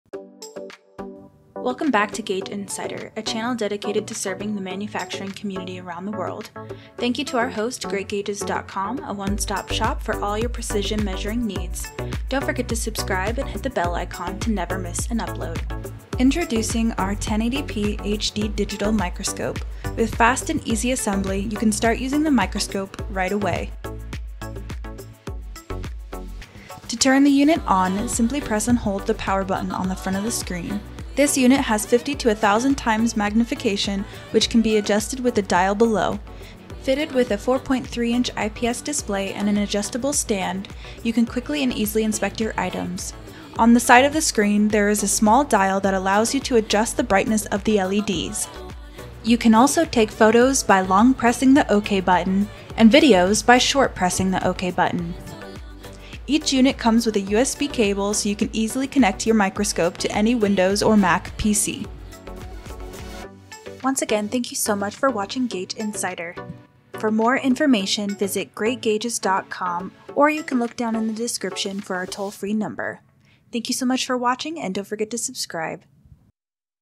How to Use & Setup 1080P HD Digital Microscope
Introducing our 1080P HD Digital Microscope. With fast and easy assembly, you can start using the microscope right away. To turn the unit on, simply press and hod the power button on the front of the screen. This unit has 50-1000x Magnification which can be adjusted with the dial below. Fitted with a 4.3 inch IPS Display, and an adjustable stand you can quickly and easily inspect your items. With an adjustable stand, you can quickly and easily move the microscope closer or further away for better viewing. On the side of the screen, there is a small dial that allows you to adjust the brightness of the LEDs.  You can also take photos by long pressing the OK button and videos by shot pressing the OK BUTTON. Each unit comes with a USB cable, so you can easily connect your microscope to any Windows or MAC PC.